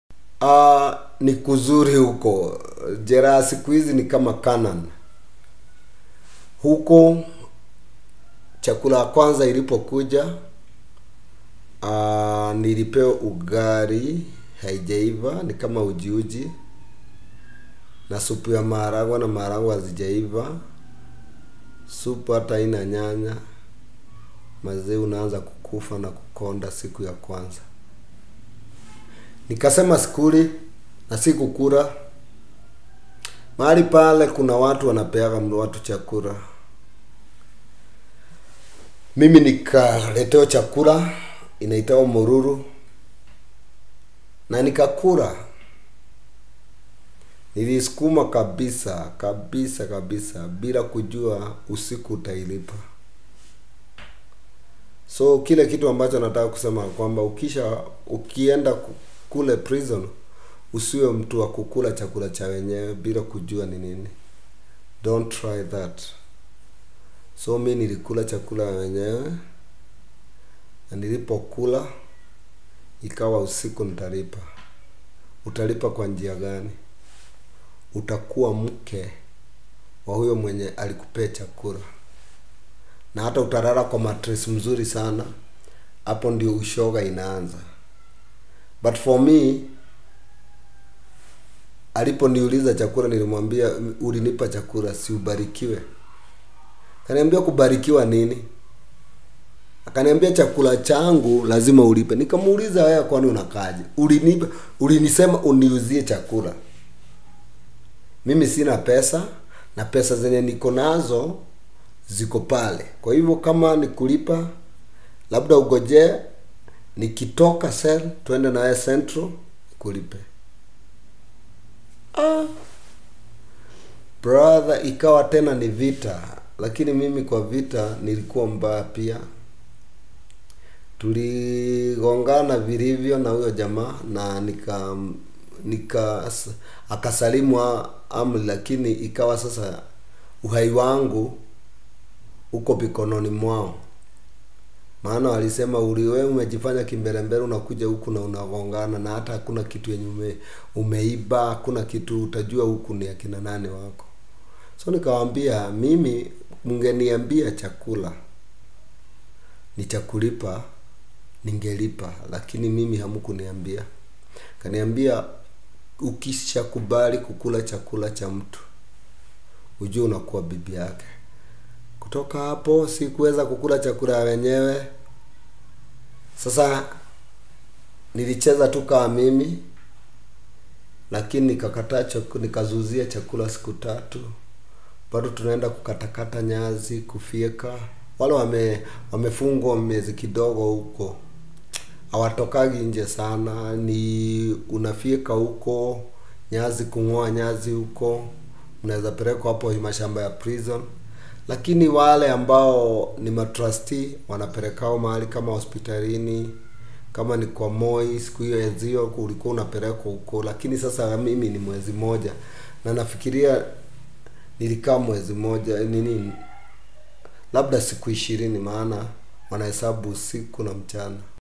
Homosexuality in Kenyan prisons | Pastor wa GenZ
Wow, now check out this snippet of the story of Pst Livingstone. Be inspired.